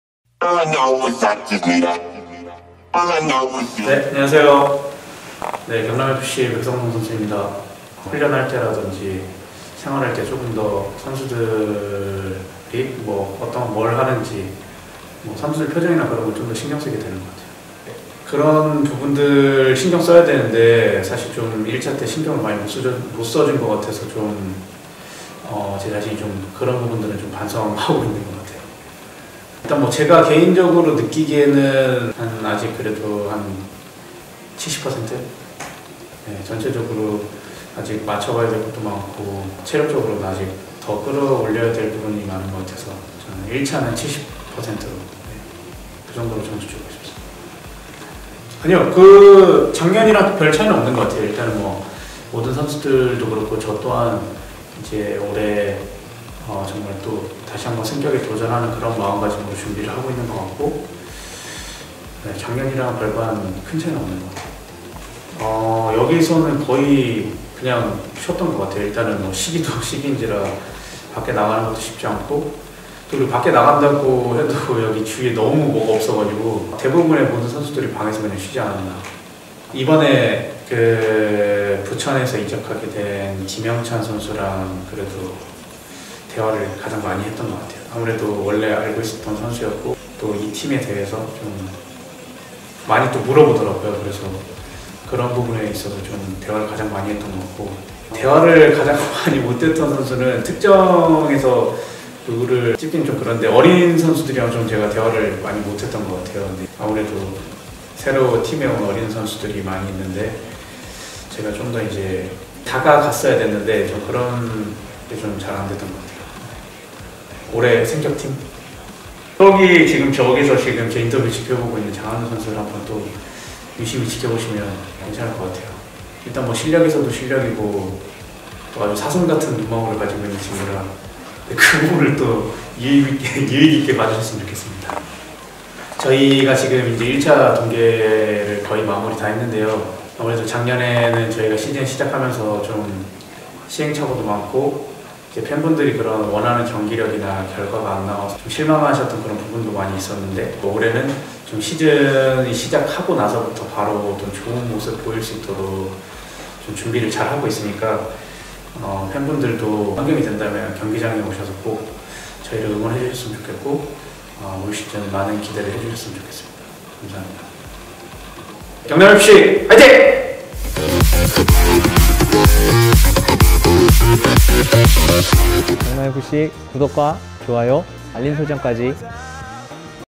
경남FC 부주장 백성동 선수 인터뷰!
경남FC 부주장 '백성동'
1차 동계 전지훈련 결산 인터뷰❤🖤
"올해는 시즌이 시작하고 나서 바로 좋은 모습 보일 수 있도록 준비를 하고 있으니까 환경이 된다면 팬분들도 경기장에 오셔서 꼭 저희를 응원해 주셨으면 좋겠고 올 시즌 많은 기대를 해주셨으면 좋겠습니다"
🔥 경남FC vs FC안양 🔥
📆 2월 27일(토) 오후 1시 30분
🏟 창원축구센터